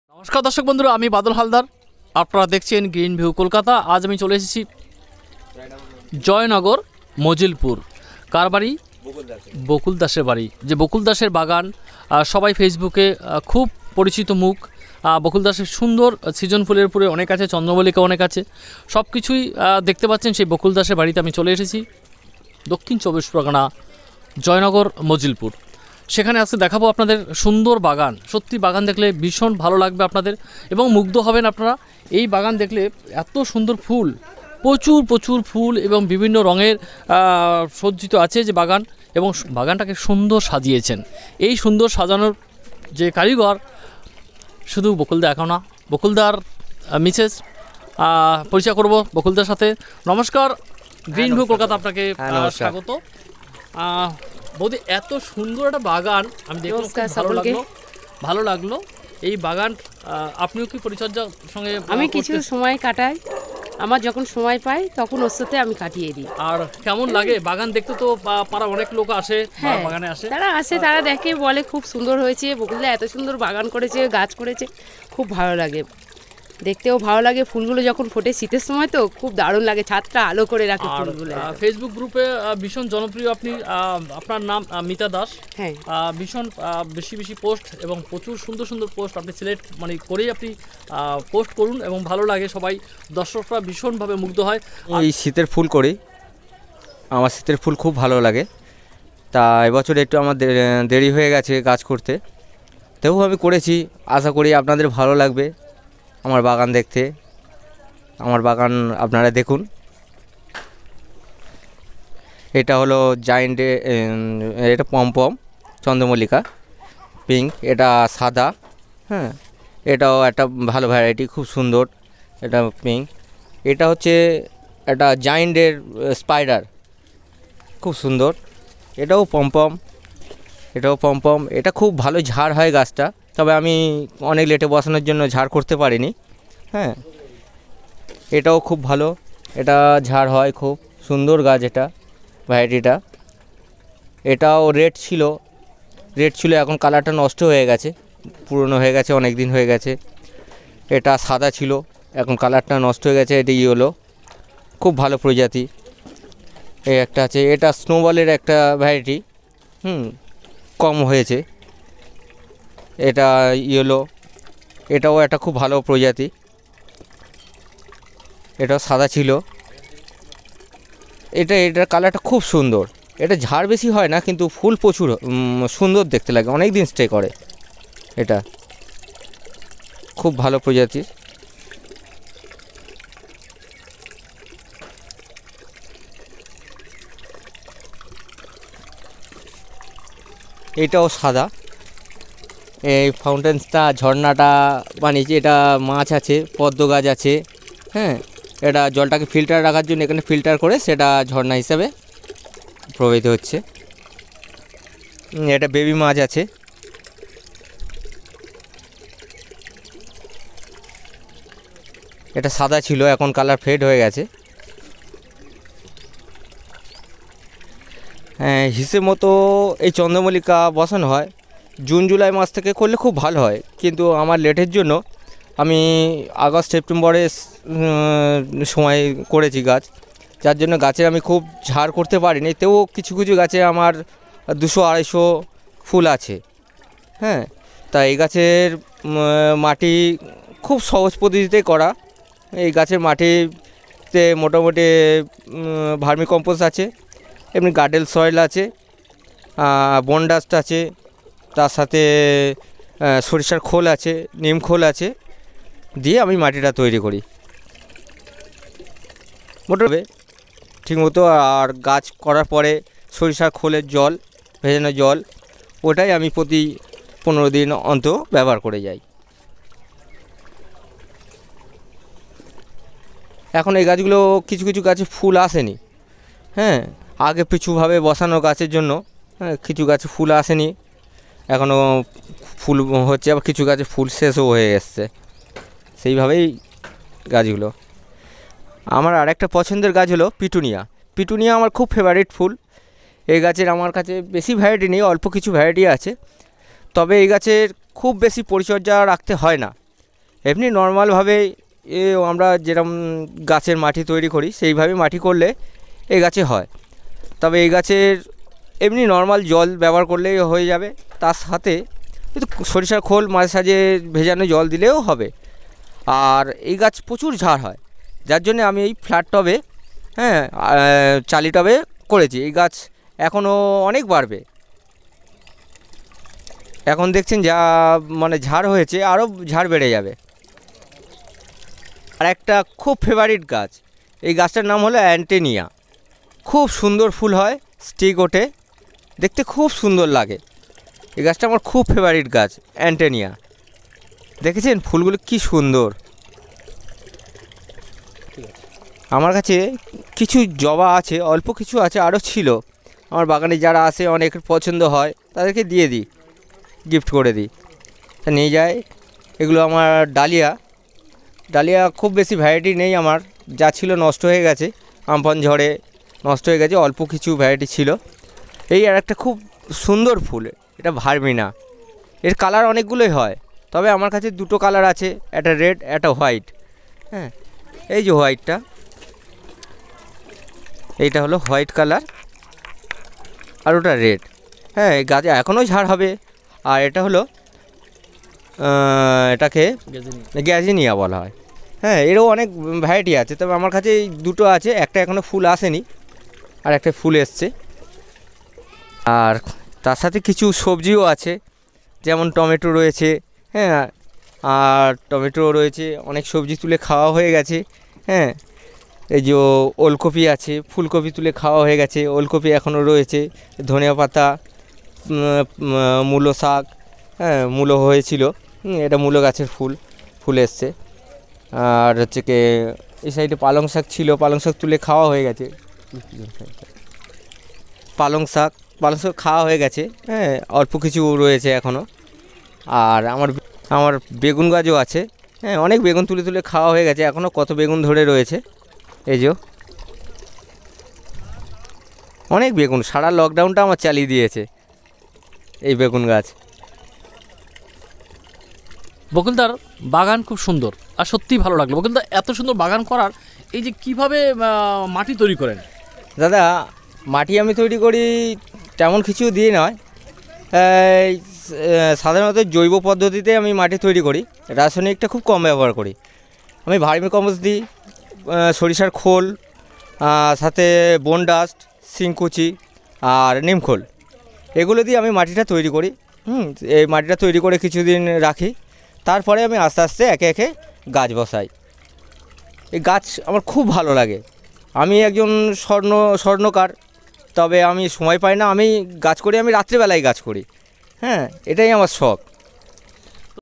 জয়নগর বকুল দার ছাদ বাগানে সাড়া জাগানো ফুলের মেলা
জয়নগর বকুল দার ছাদ বাগানে ফুলের মেলা সত্যিই এই মেলা দেখার মত নানারকম ফুলে সজ্জিত বাগান গ্রীন ভিউ কলকাতা চ্যানেলে তুলে ধরা হয়েছে এত সুন্দর বাগান কিভাবে করবেন সকল তথ্য তুলে ধরেছেন বকুল দাস এবং নানা রকম ফুল দিয়ে সাজিয়েছেন নিজের বাড়ির ছাদের চারদিকে সবুজ প্রিয় বন্ধুদের আকর্ষণ করবে। আপনারা যদি এরকম ছাদ বাগান থাকে যোগাযোগ করতে পারেন এই চ্যানেলে আমাদের নাম্বার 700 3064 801